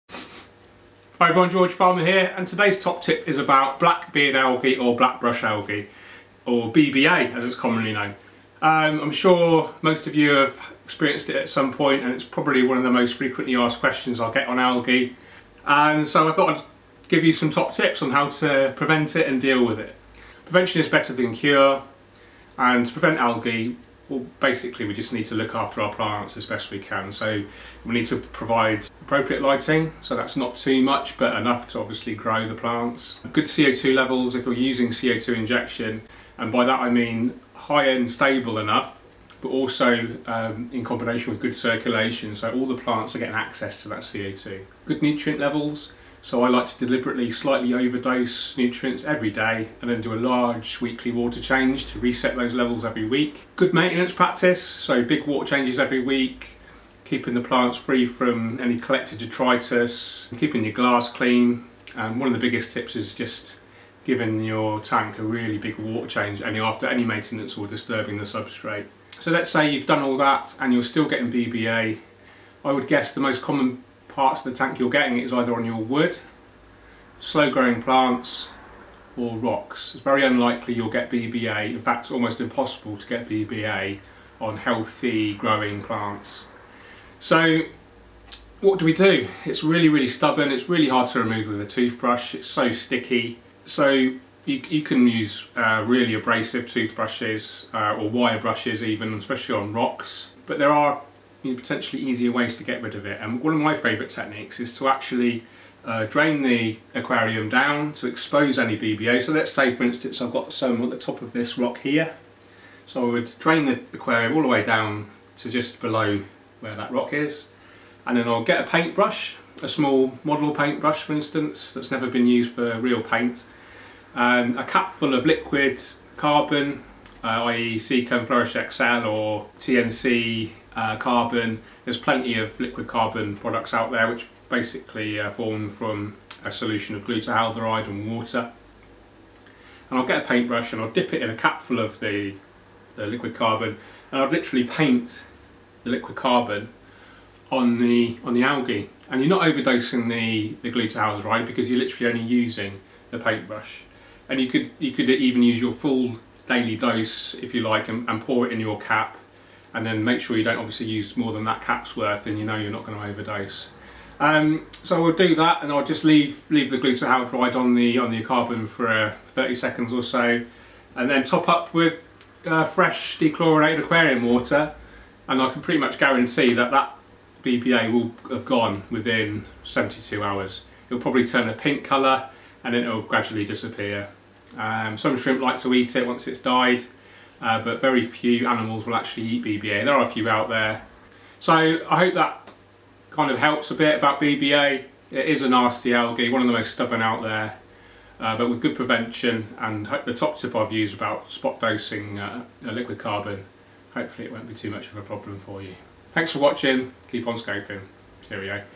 DEFEAT BLACK BRUSH ALGAE NOW using my simple technique
This video explains how to prevent and treat the dreaded black beard or black brush algae.

Please treat any GLUTARALDEHYDE-based product with respect! It's very toxic.

Keep on 'scaping,
George